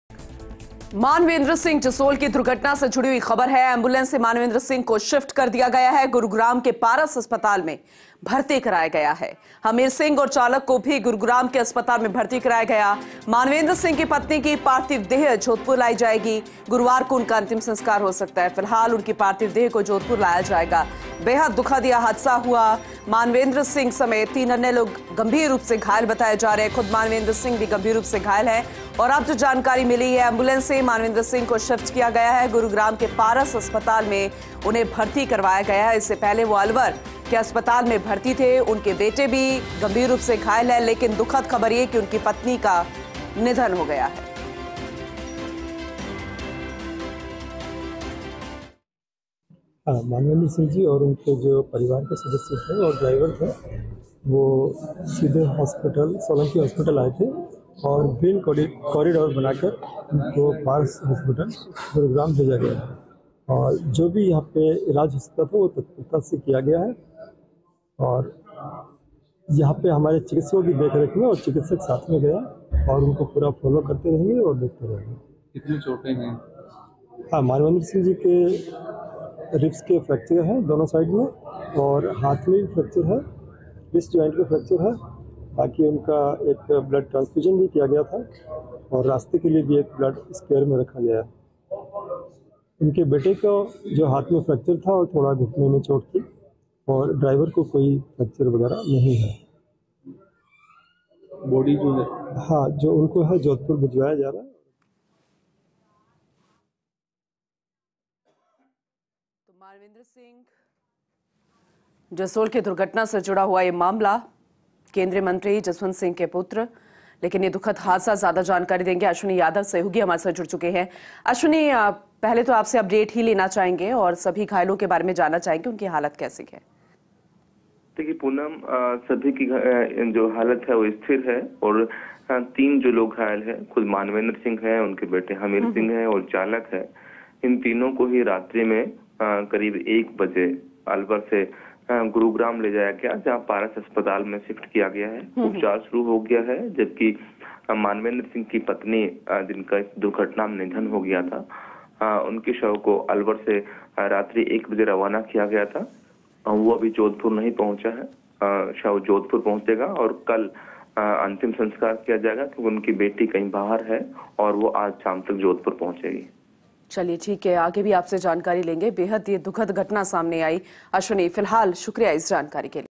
Manvendra Singh Jasol को गुरुग्राम के पारस अस्पताल में कराया गया भर्ती | Alwar News
About this Video: Manvendra Singh Jasol को गुरुग्राम के पारस अस्पताल में कराया गया भर्ती | Alwar News | 31 January 2024